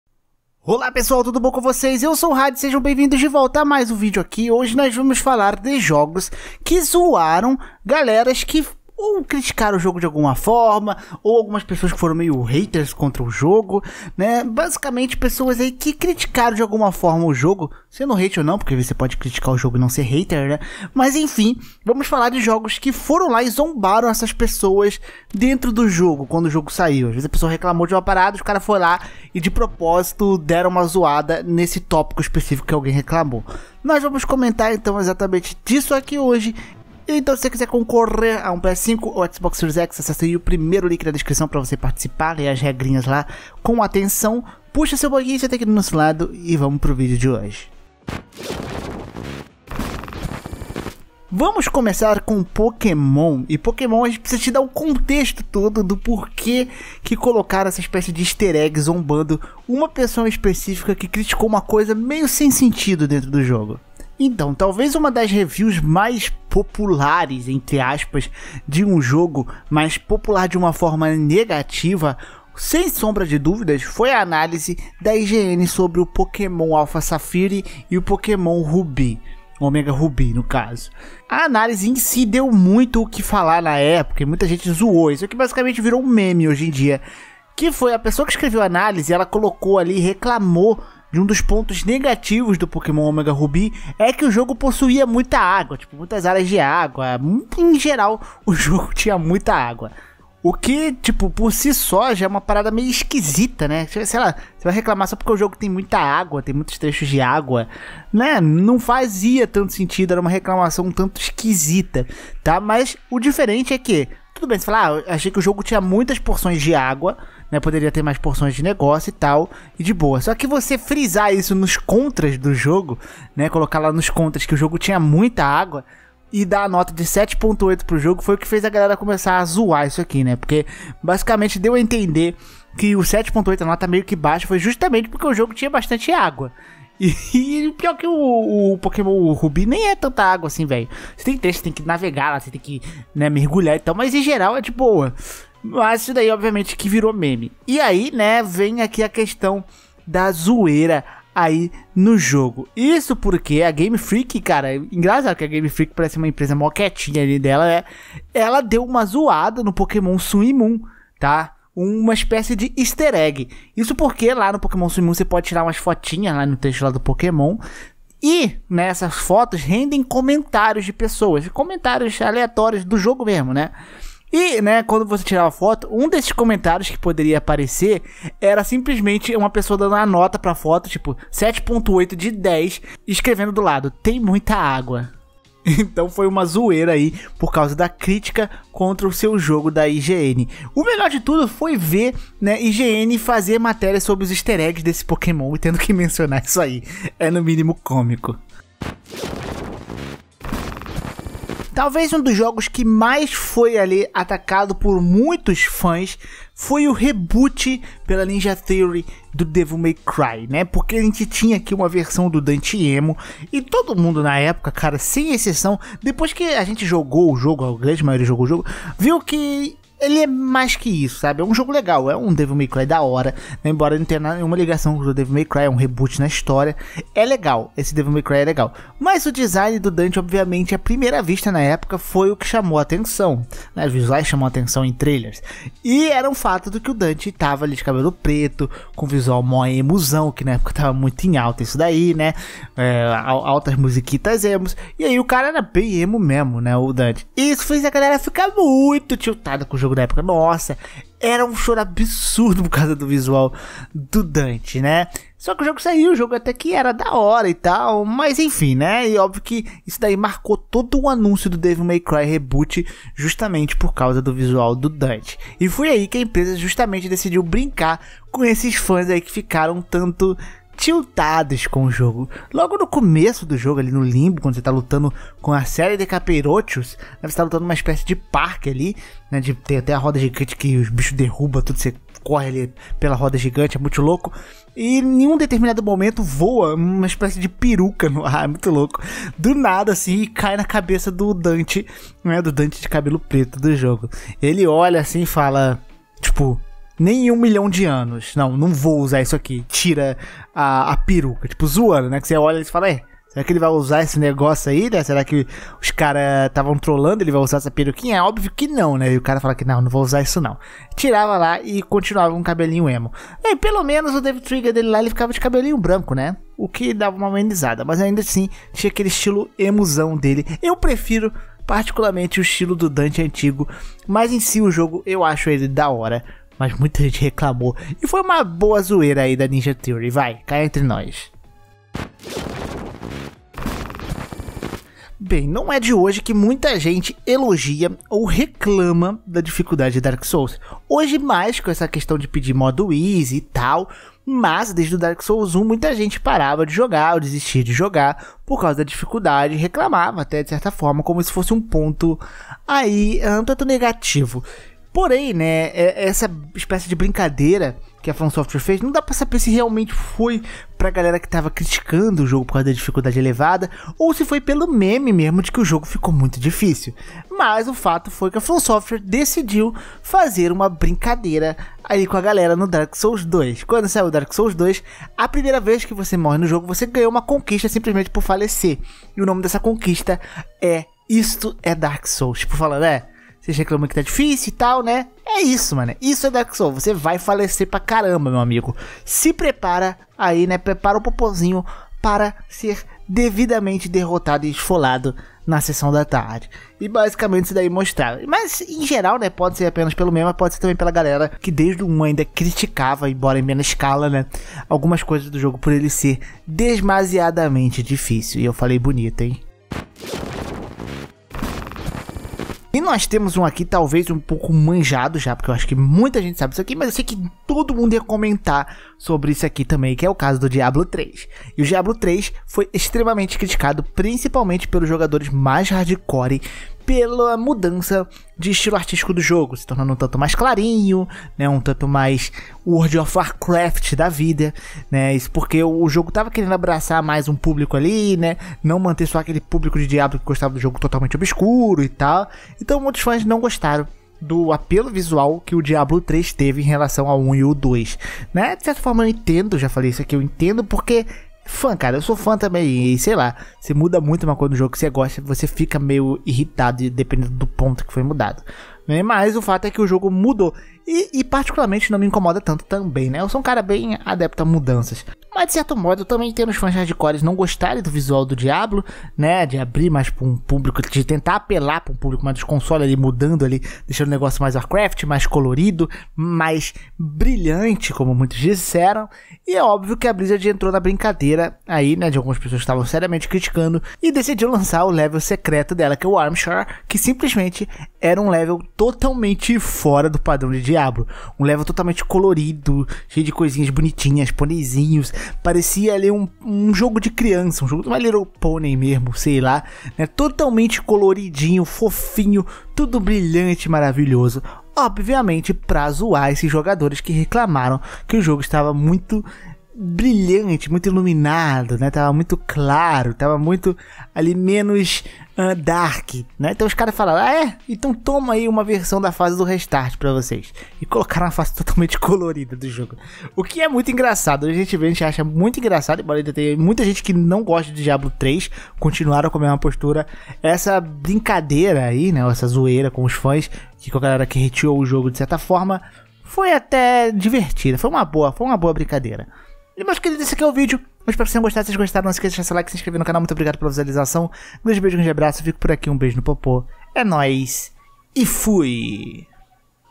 Jogos que ZOARAM a cara dos seus HATERS!
Jogos que brincaram com quem reclamou!

➤ PSN: hadessama94